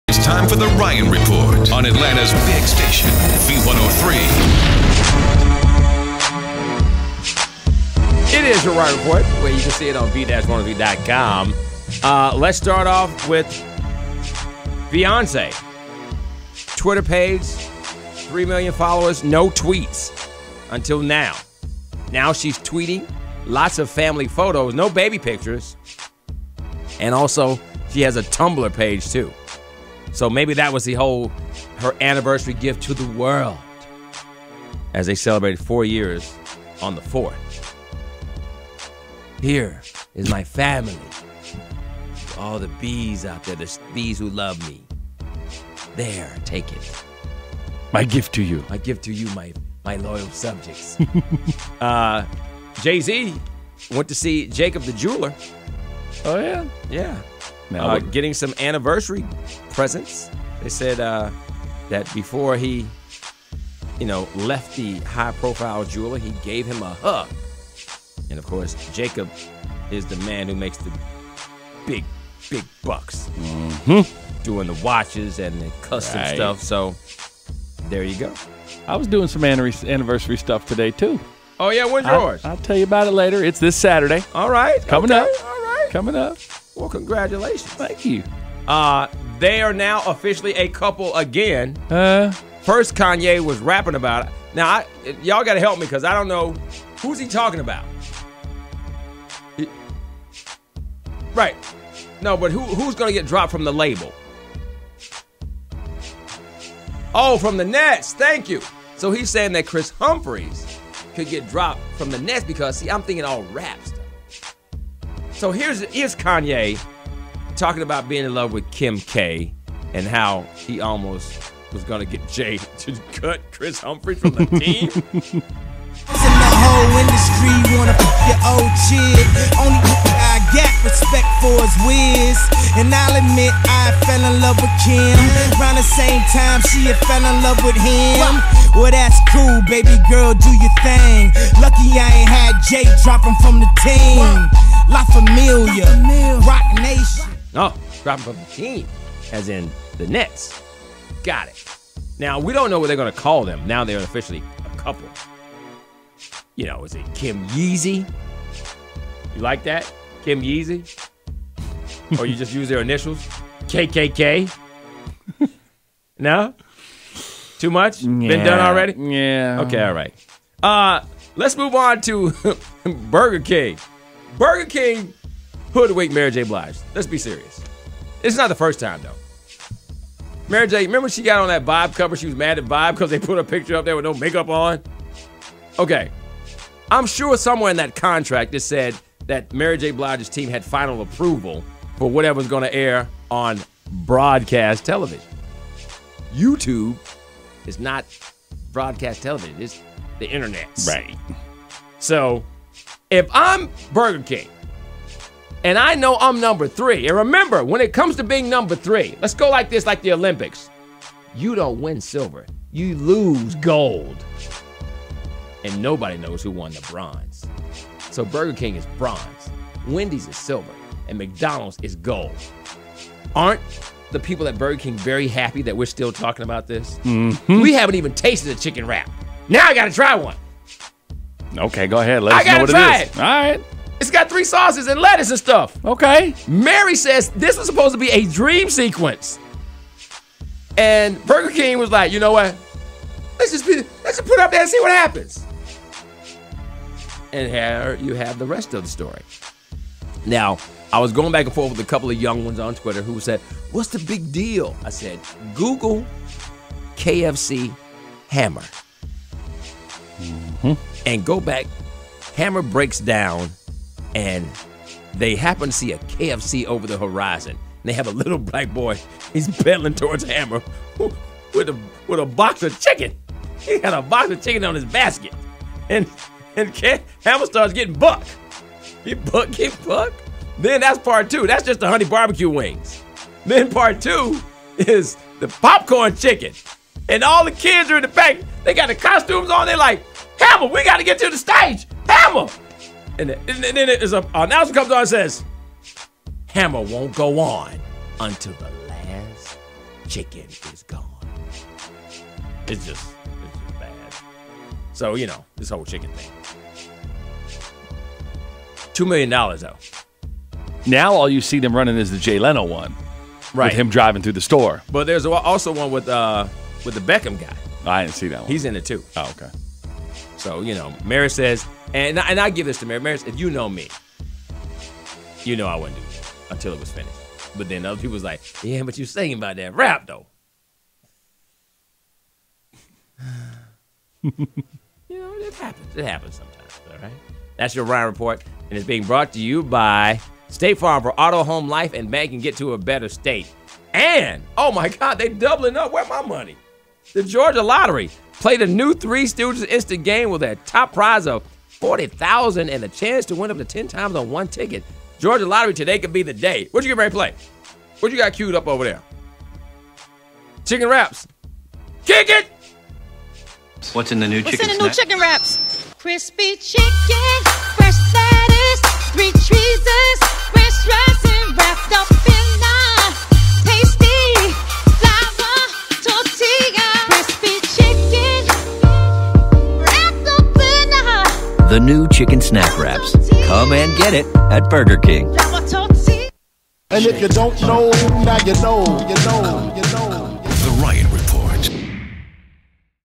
The Ryan Report: Was Mary J. Blige Deceived? 04/05/12
Today on the report:
• Beyonce breaks her Twitter silence
• Find out where Jay-Z shopped for their anniversary
• What do you call Kanye West and Kim Kardashian now that they are back together?
• Did Burger King hoodwink Mary J Blige?